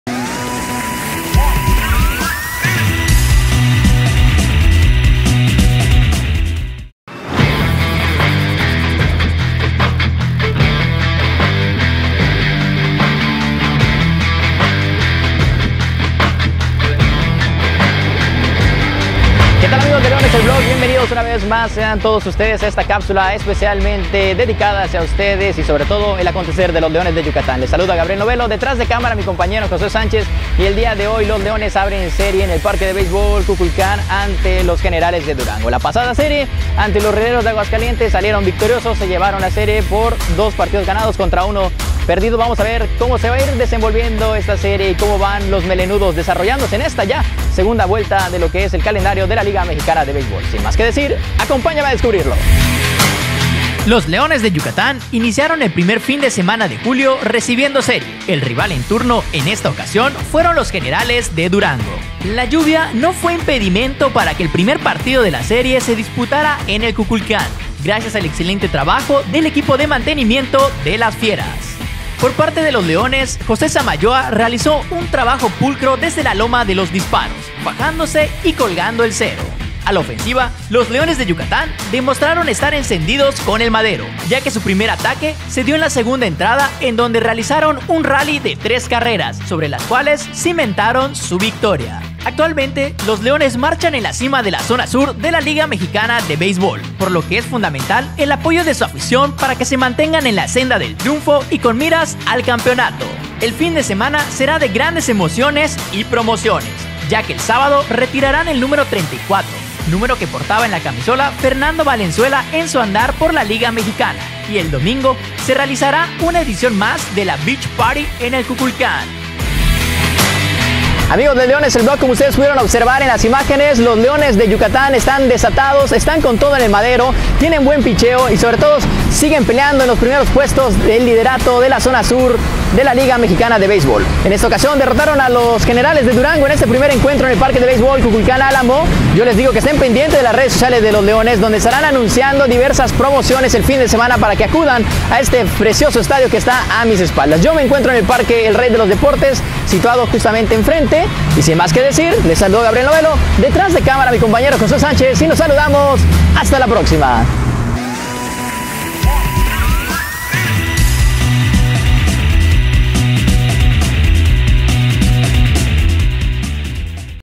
Festival de macanazos en el Kukulcán - Leones vs. Generales
Un festival de macanazos en el estadio Kukulcán Alamo, los Leones de Yucatán vencieron 9-0 a los de Generales de Durango. Así es como se vivió ⬇️⚾️🦁